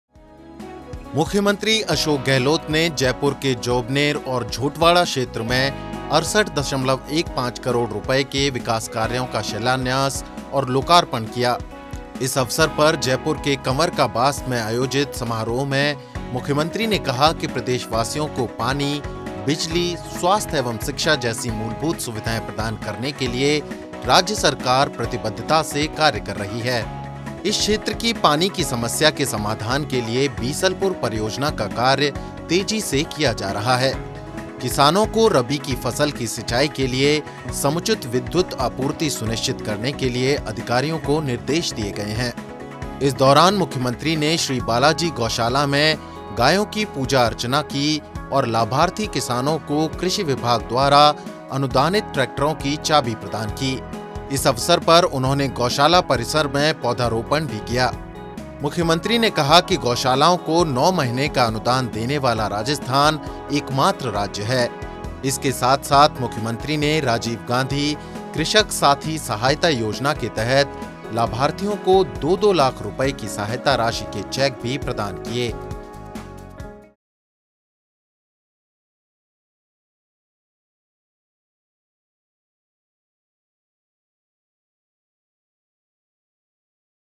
Rajasthan Govt committed to provide essential facilities: CM Gehlot
Chief Minister Ashok Gehlot laid the foundation stone and inaugurated several developmental works worth Rs 68.15 crore in the Jobner and Jhotwara areas of Jaipur. On this occasion, the Chief Minister said that the state government is working with commitment to provide basic facilities like water, electricity, health and education to the people of the state. To solve the water problem of the area, the Bisalpur project is underway at a fast pace. Instructions have been given to the officers to ensure adequate power supply to farmers for irrigation.